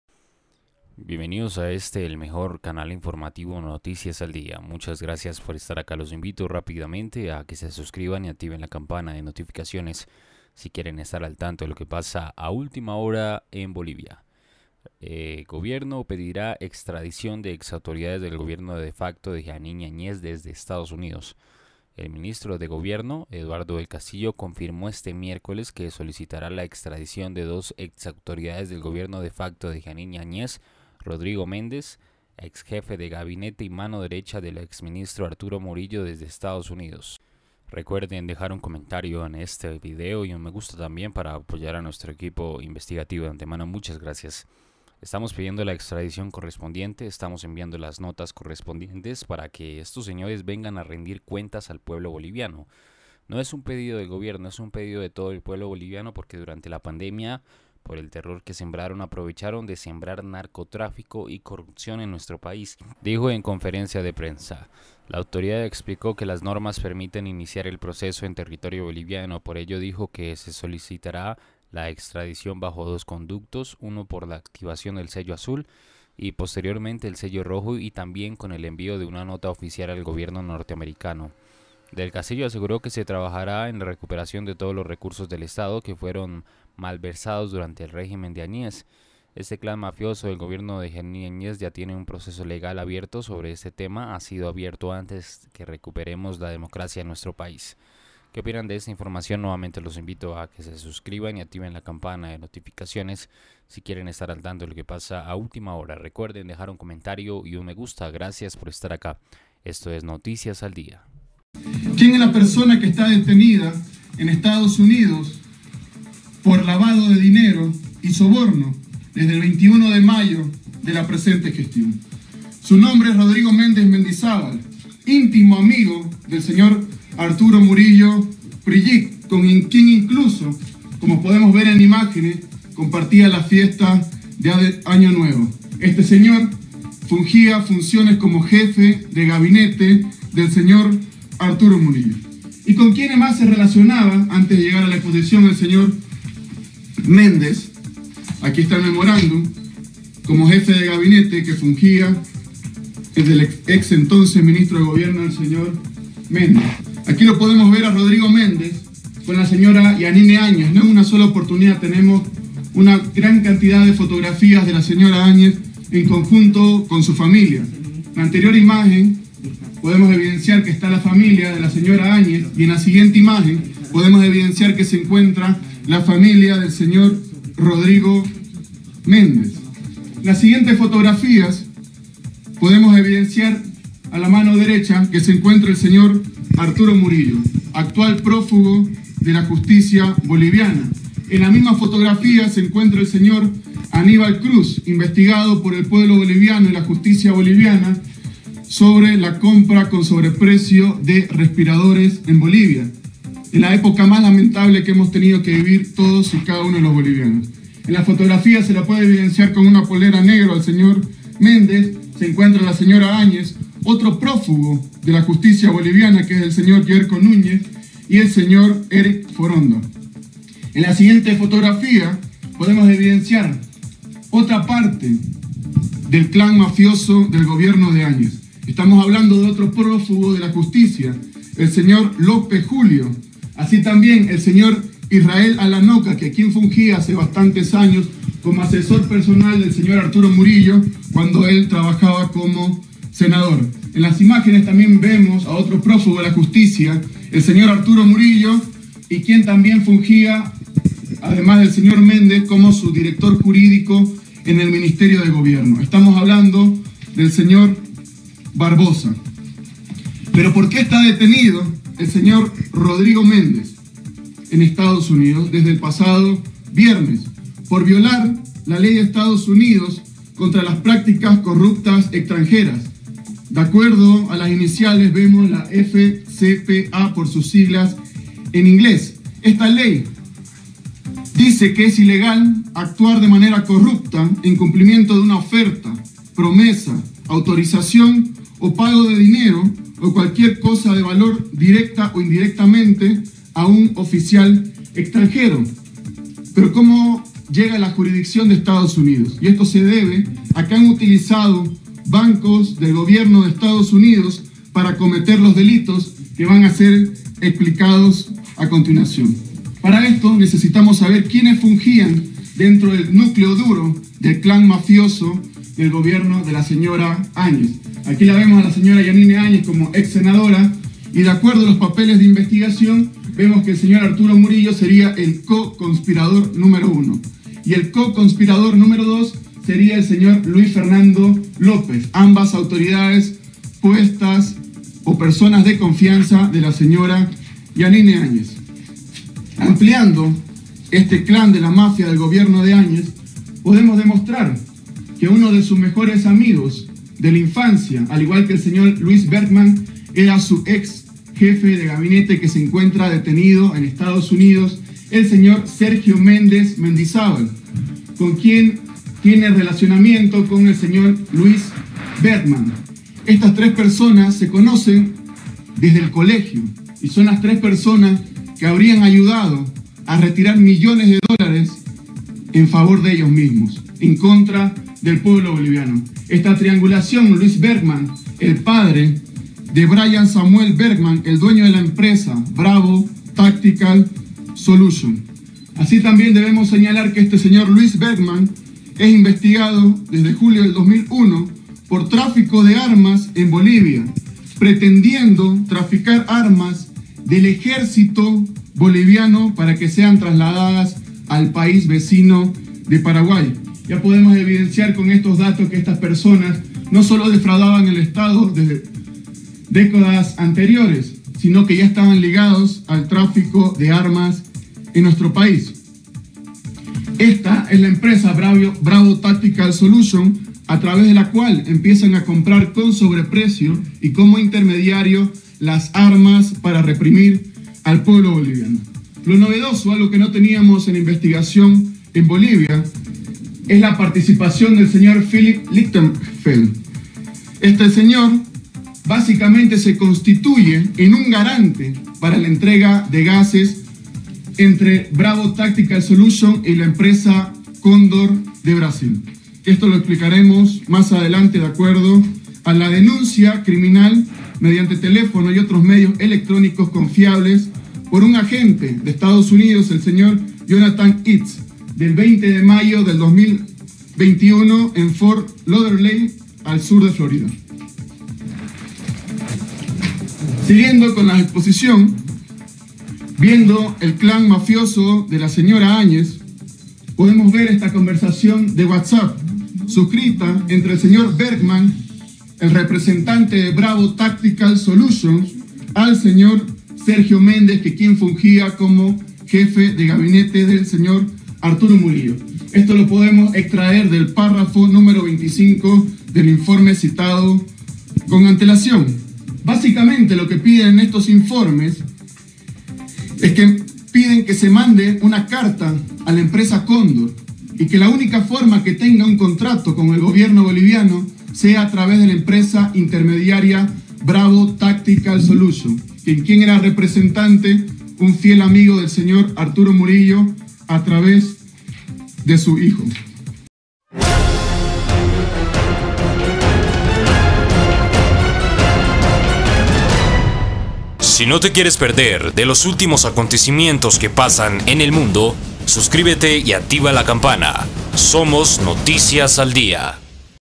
BOLIVIA HOY NOTICIAS 26 DE MAYO 2021, JEANINE AÑEZ R3CHAZADA POR EL GOBIERNO INVIERTE SEGURO
Gracias!

ultima hora bolivia hoy 26 de mayo 2021
noticias de bolivia hoy 26 de mayo 2021